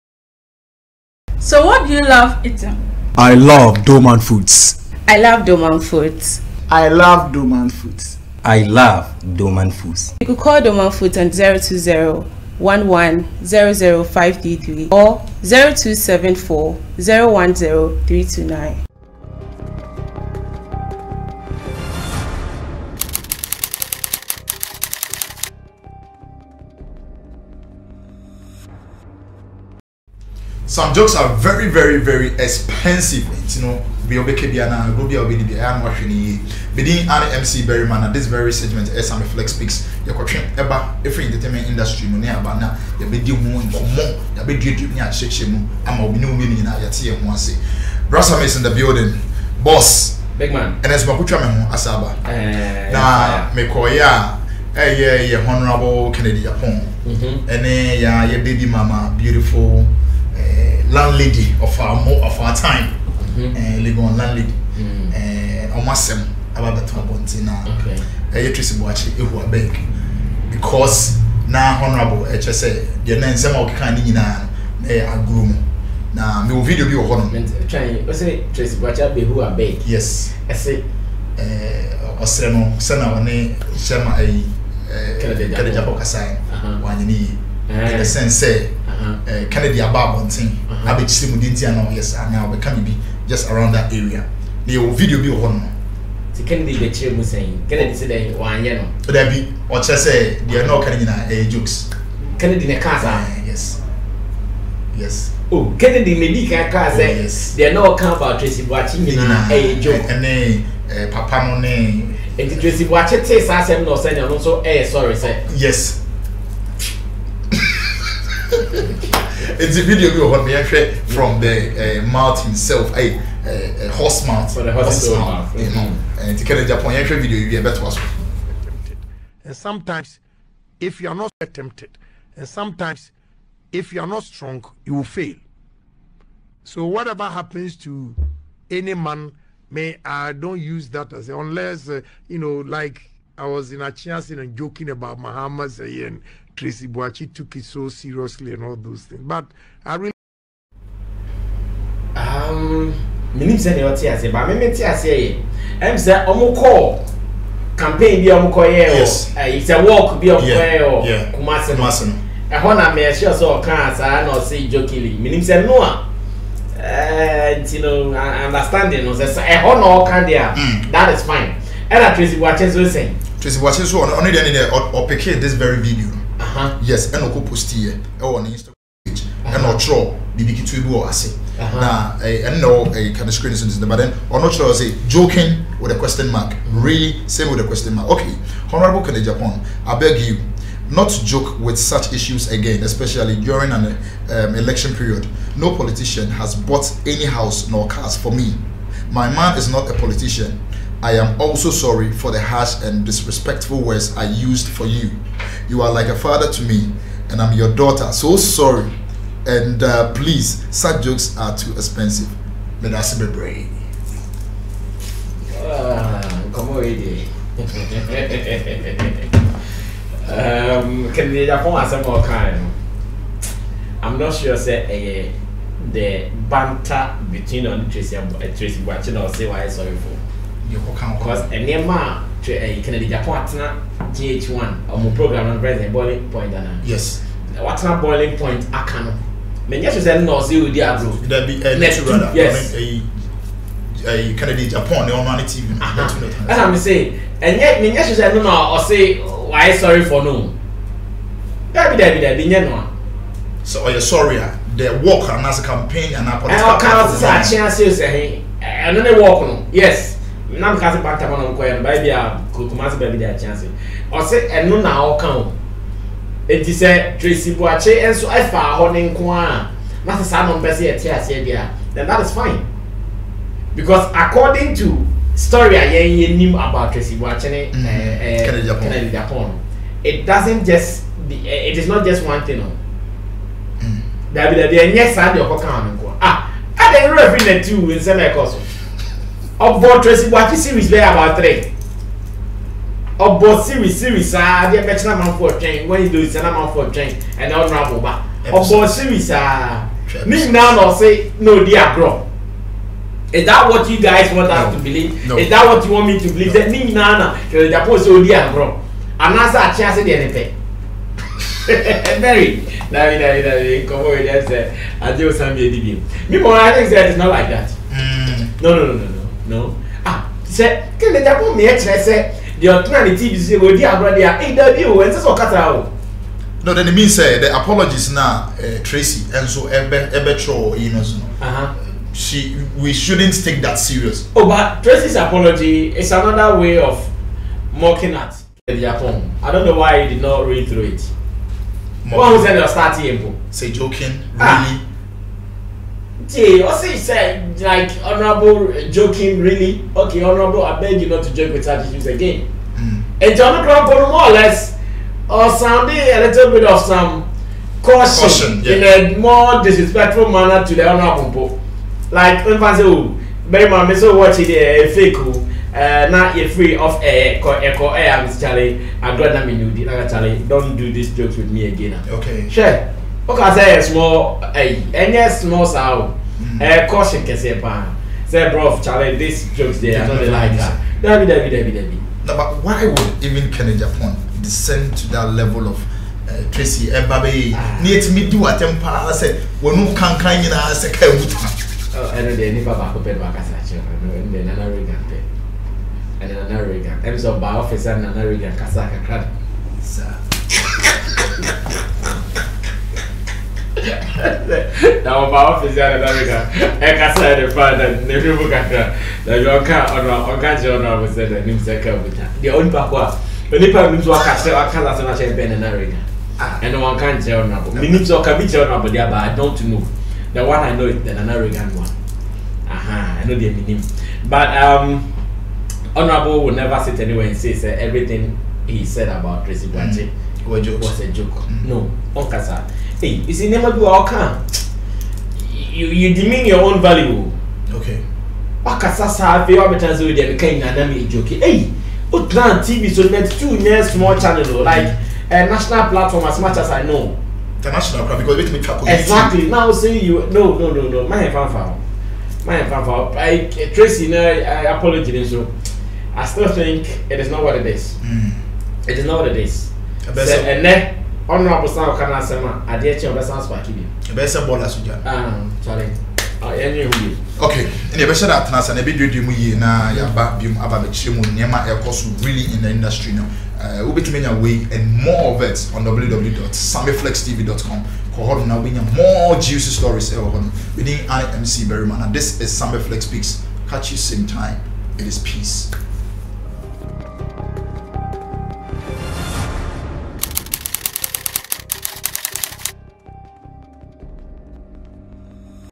Fake Apologies From Tracy Boakye vrs Hon. Kennedy Agyepong: Sammy Flex Tells Why & How
A compilation of stories making headlines in and outside our industry powered by Flex Entertainment.